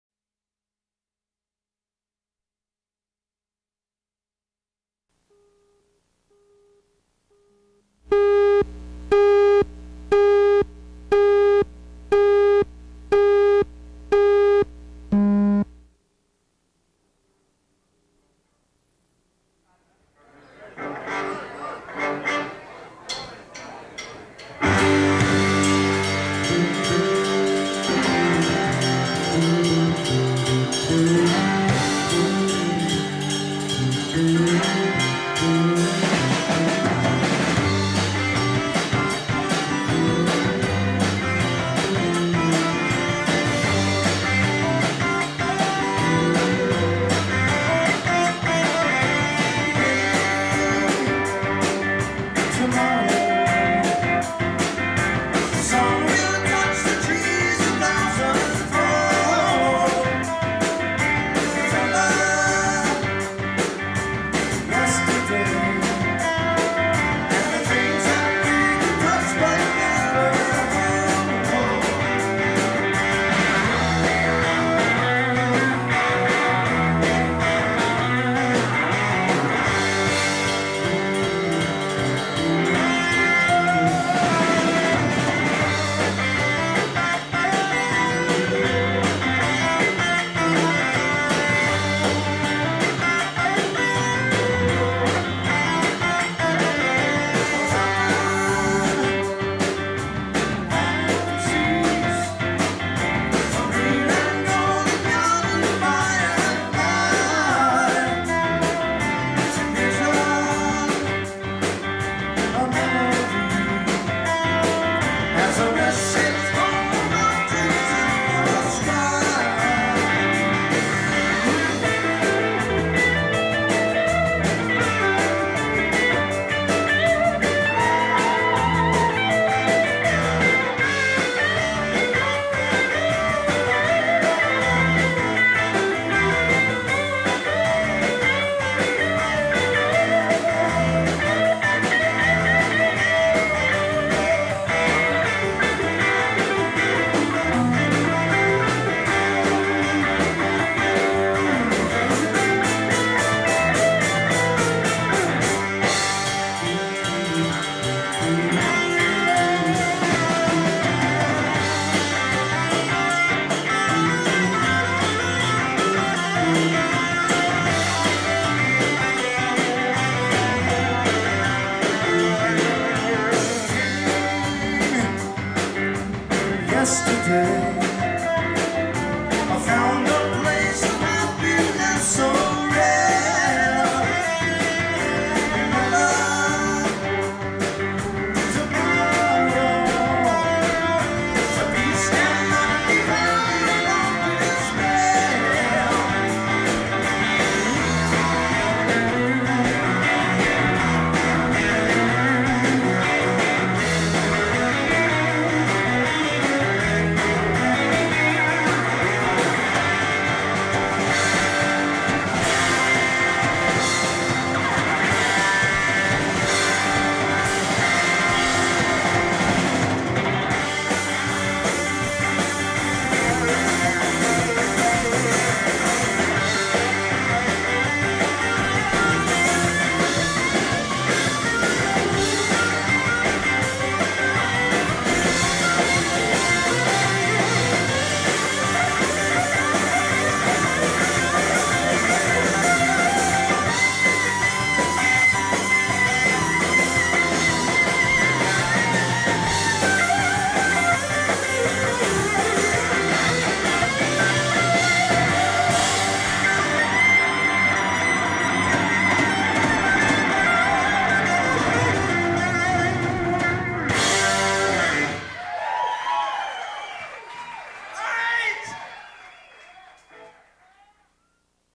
Bubble Puppy Part 1
Forgive the state of the art video editing system at ACTV in 1986. philmorvideo #27 Bubble Puppy at the Continental Club Austin 1986.